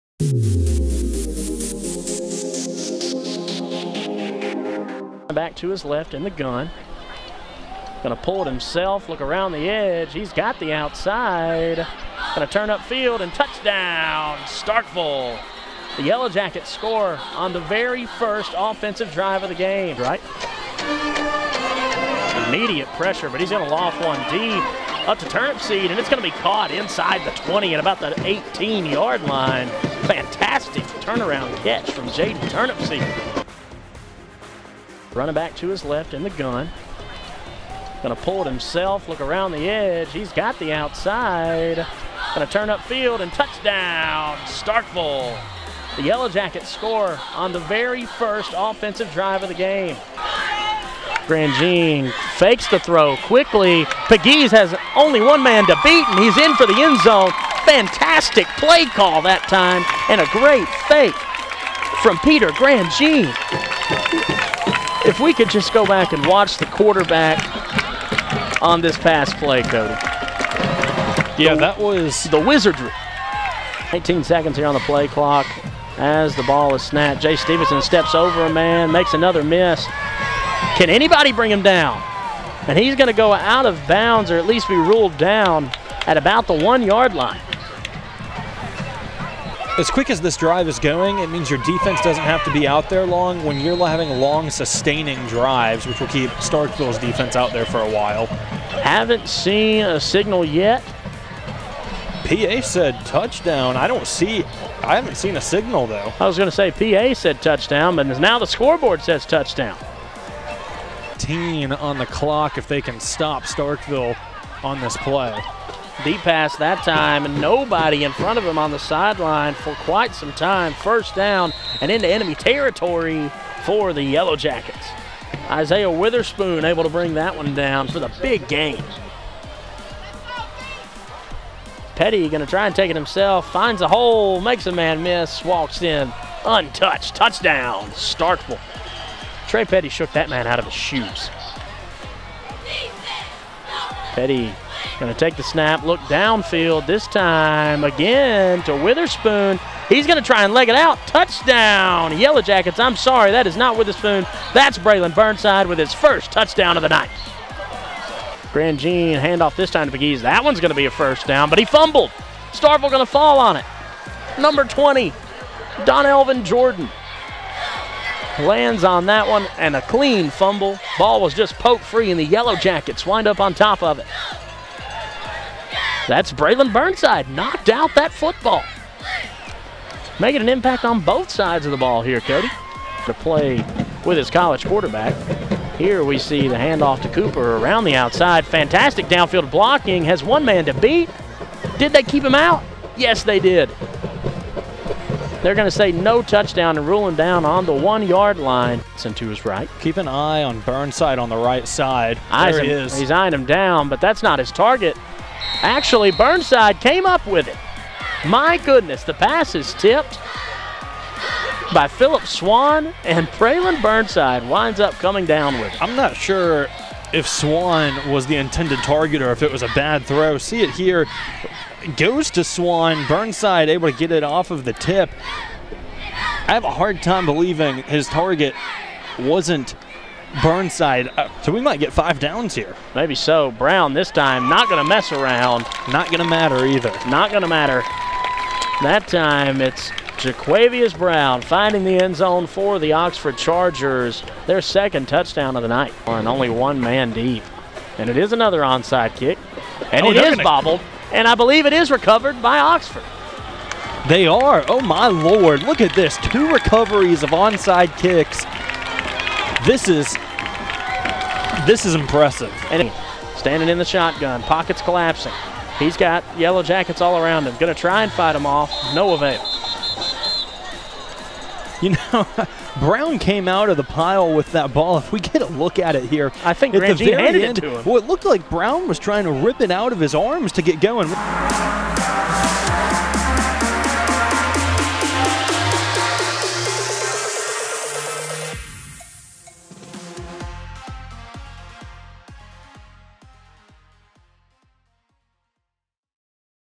Oxford VS Starkville Game of the Week Highlights
The Starkville Yellow Jackets got the win in the Little Egg Bowl over the Oxford Chargers in 7A Football for the Game of the Week. Catch Football all season on Saturdays at 7:00 p.m. on channels 31 & 1031 exclusively aired on @MaxxsouthBroadband